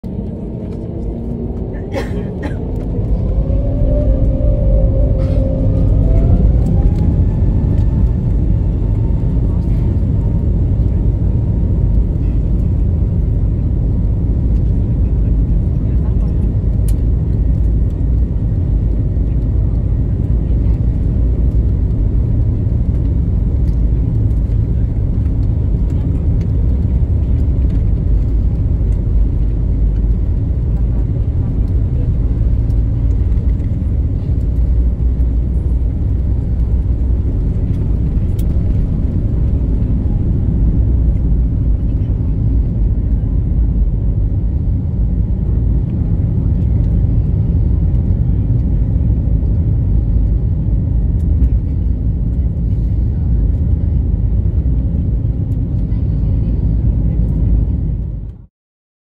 Aircraft speed check during takeoff | Indigo Hyderabad to Bangalore flight
Flight speed check on Indigo 6E 6417 from Hyderabad to Bengaluru flight at night. The speed rises from 0 to around 250 kmph during take off.